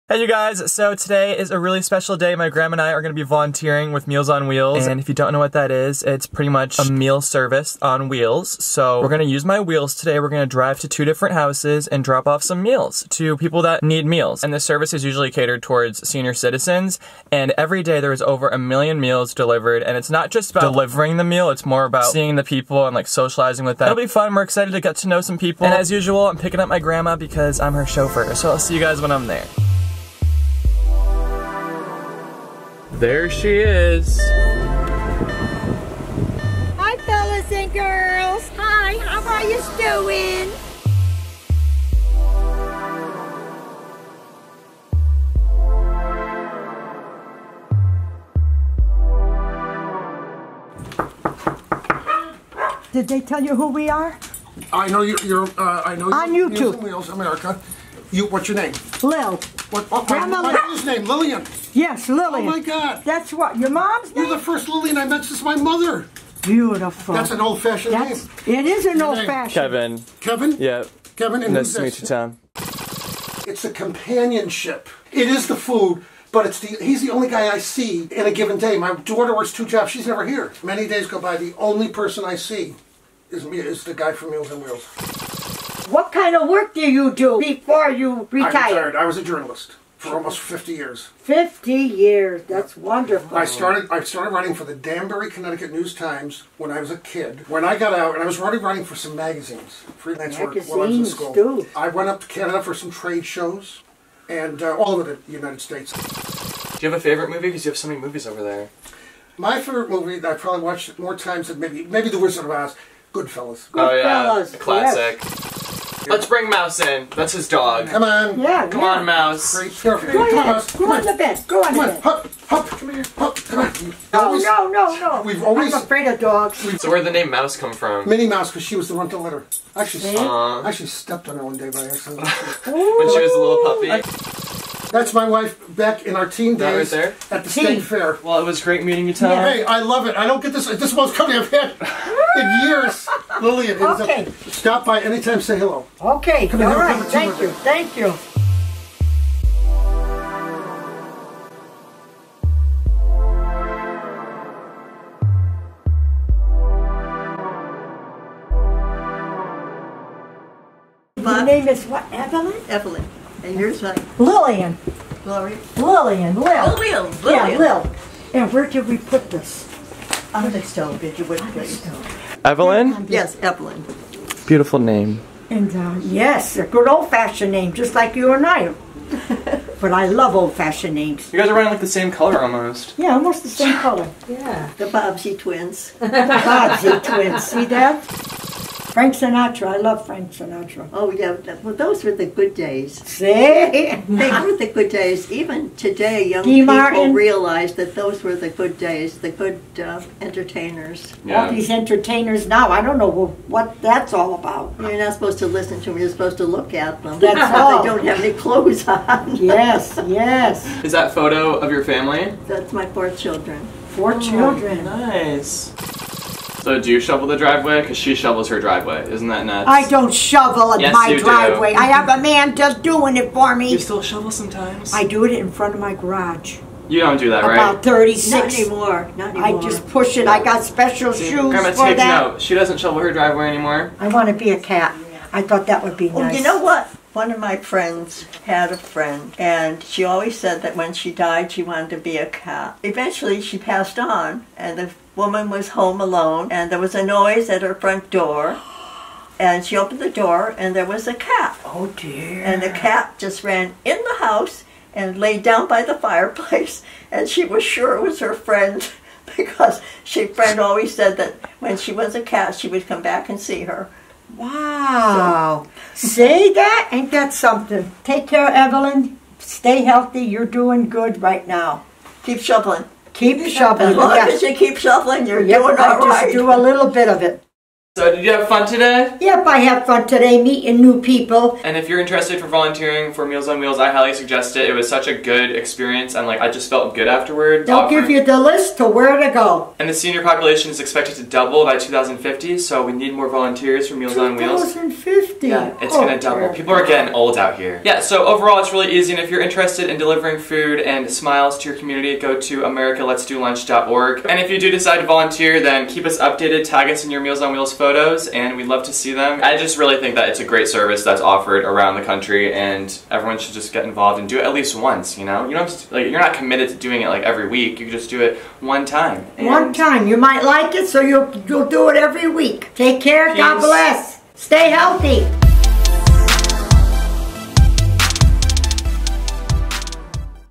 Volunteering With My Grandma (Meals On Wheels)
We had so much fun doing this and I hope you enjoyed watching! 🍛🚗
To donate your lunch break and spend some time with amazing seniors, check out